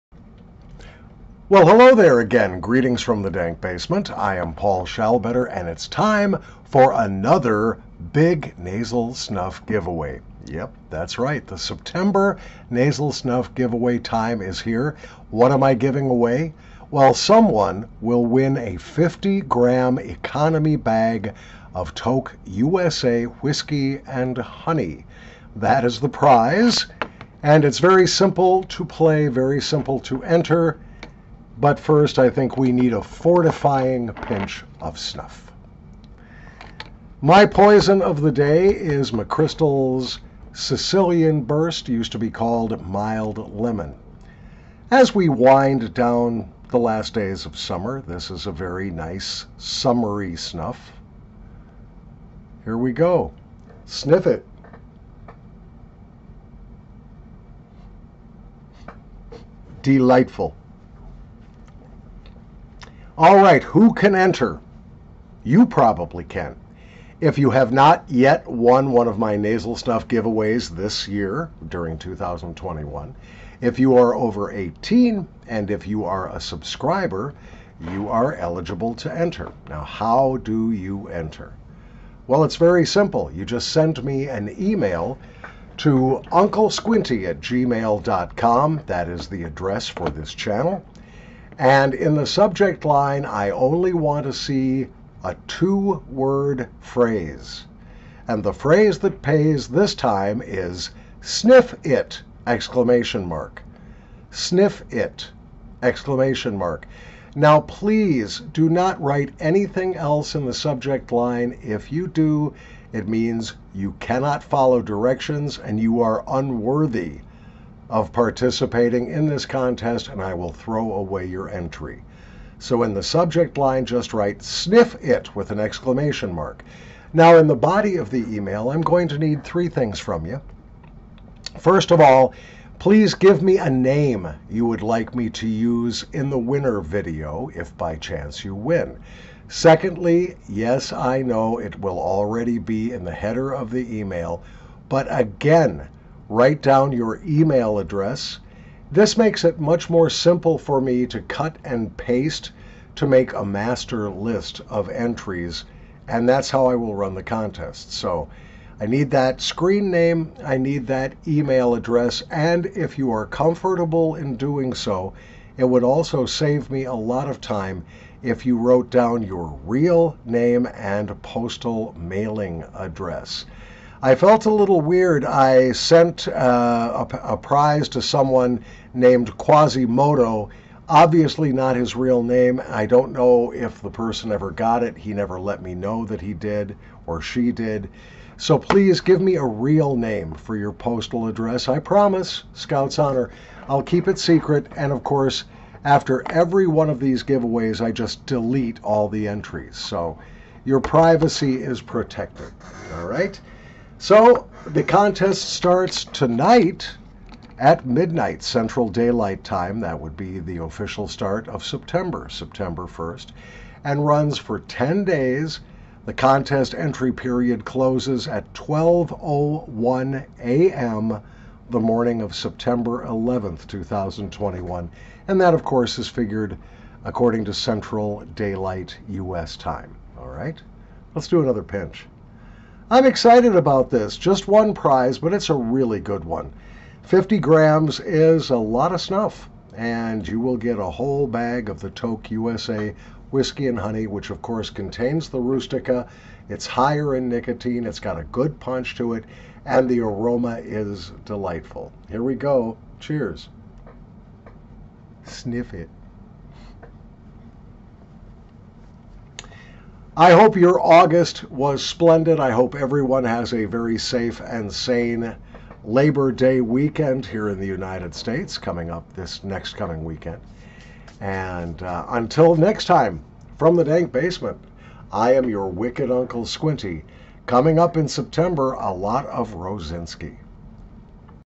Big September Nasal Snuff Giveaway! August 31, 2021
This contest is open to anyone who has not won one of my giveaways druing 2021, who is over 18 and who is a subscriber to this channel!  The giveaway is open to anyone living on this big blue marble called Earth.

The rest of the rules are explained in the video.  Listen carefully!  The contest opens tonight at 12:00 midnight U.S. Central Daylight Time and closes at 12:01 a.m. on September 11, 2021, Central Daylight Time.